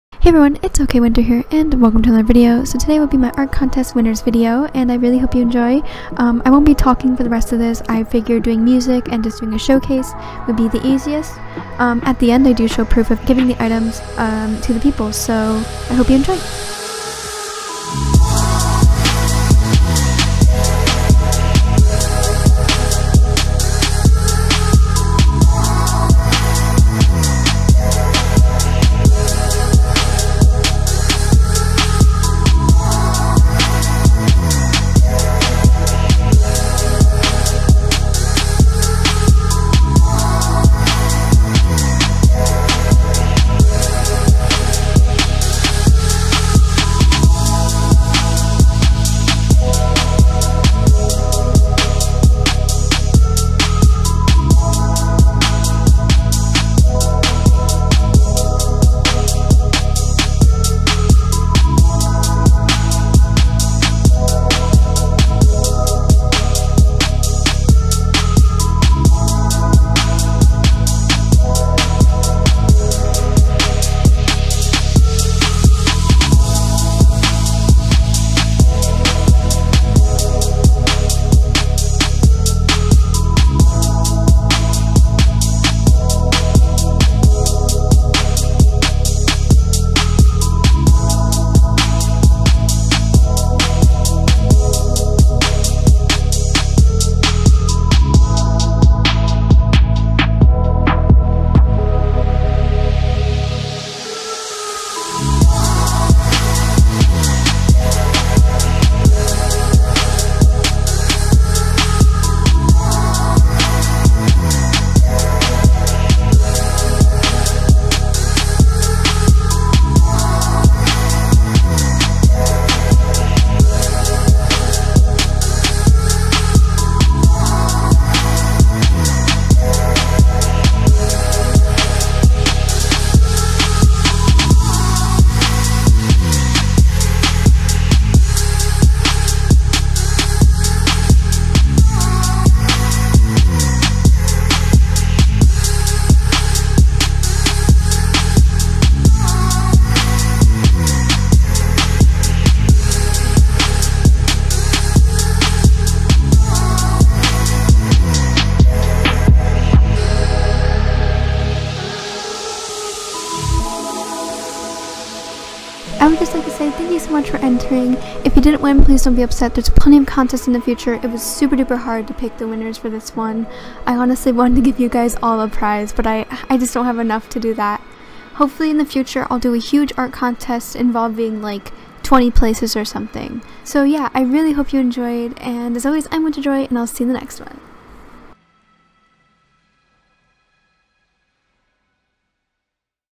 ANIMAL JAM ART CONTEST WINNERS
PLEASE READ!!
IM SO SORRY THIS TOOK SO LONG TO MAKE AND RENDER!!
..but I would like to thank everyone for entering!! you all did so amazing and I appreciate every single one of u!! ♡♡♡

- IF I FORGOT TO SHOWCASE UR ENTRY ITS BECAUSE THIS VIDEO IS RUSHED AND I TRIED TO PUT EVERYONE IN HERE IT WAS HARD TO MAKE FJHGLKDJFHG

♡ - Information - ♡

♡ Recording software is Bandicam or Quicktime

ʕ•́ᴥ•̀ʔっ